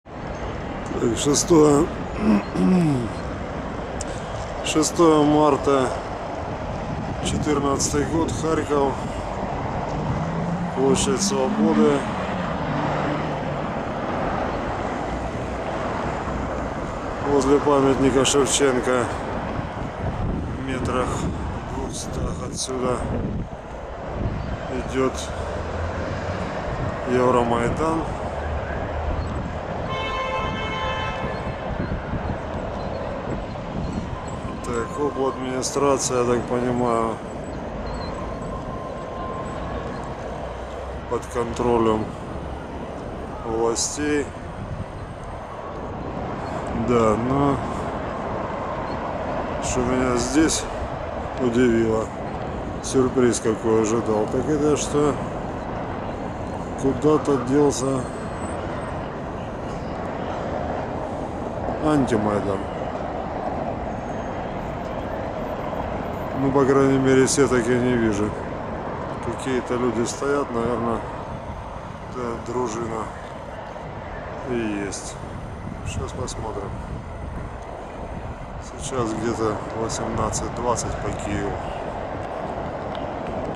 Милиция возле обладминистрации Харькова, 06.03.14., пл. Свободы
После вчерашнего противостояния в Харькове пророссийских сил и милиции -- площадь Свободы пуста на 18.20. шестого марта 2014 года.
Кордон милиции возле харьковской областной администрации. Памятник Ленину освобождён от защитных ограждений.